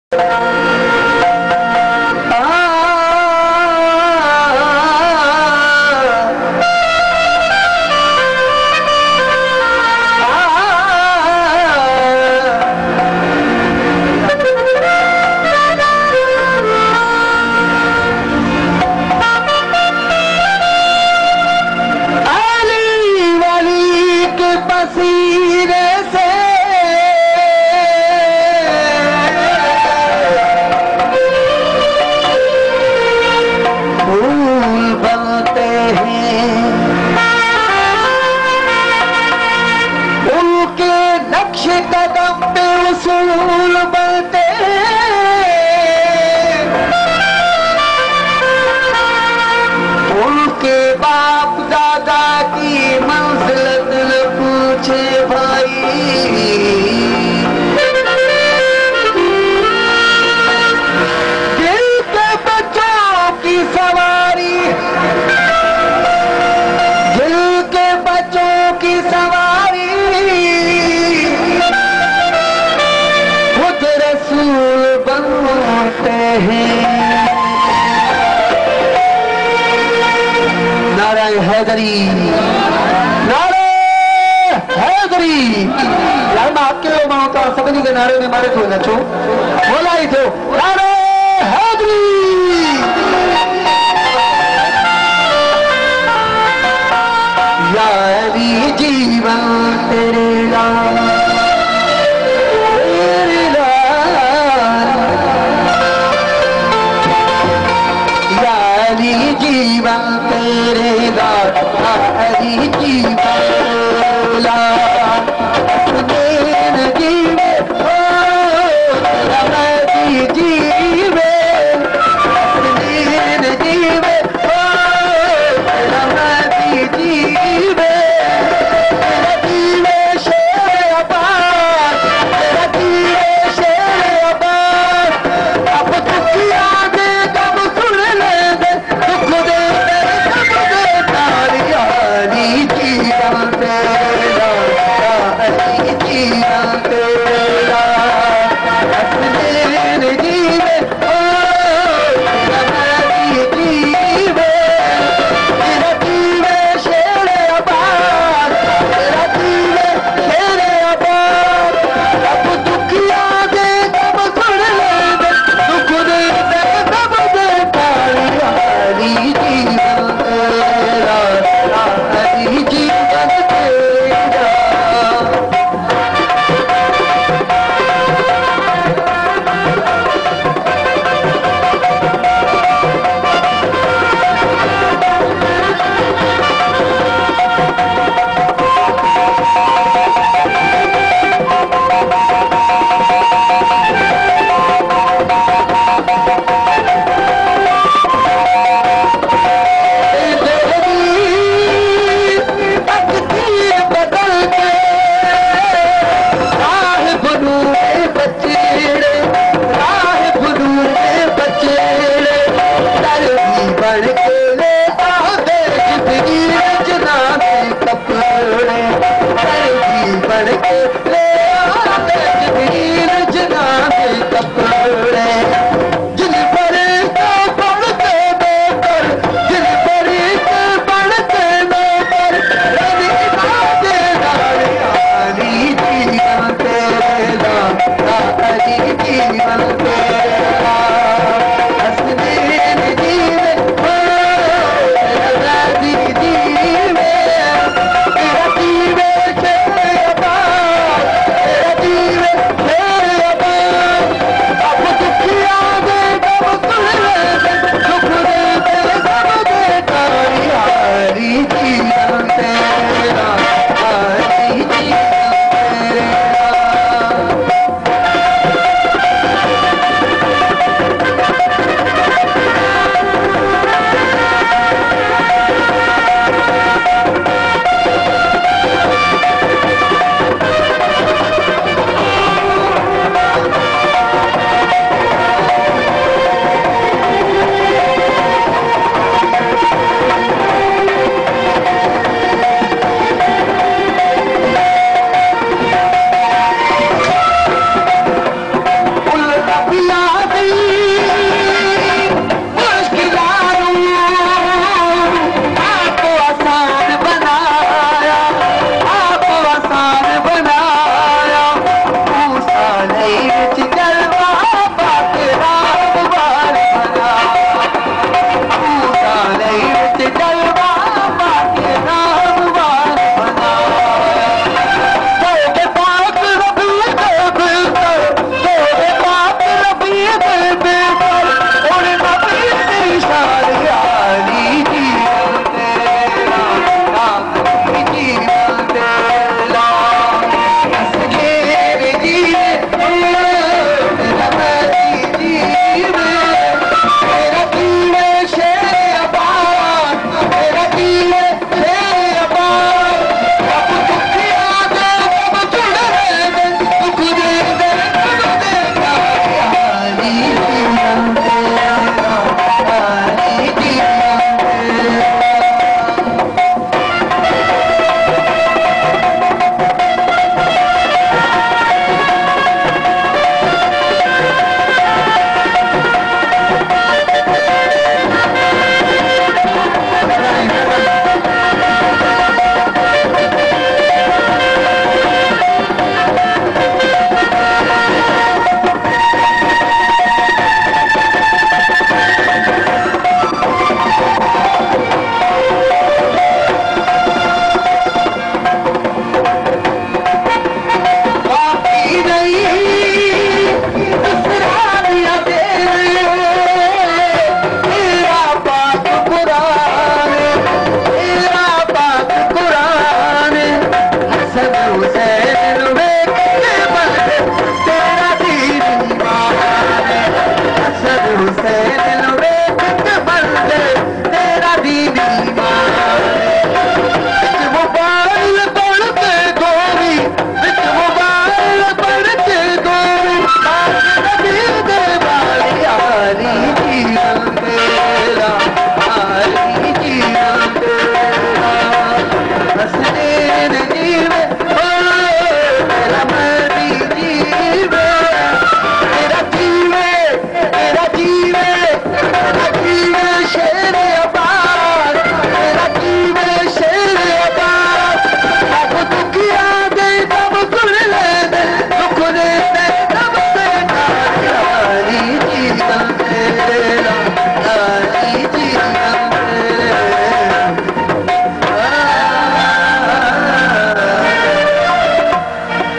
jashn e moled e kaiba hazerat mola ali A S by 2010(Munawar Ali jamshoro)
jashn e Moled kaiba Hazerat Mola Ali A S 2010 (vilej Paka Chang Bherya Rod)
Recodeg by (A khaleQ meman Gulam rabani & Munawar Ali Bhounr)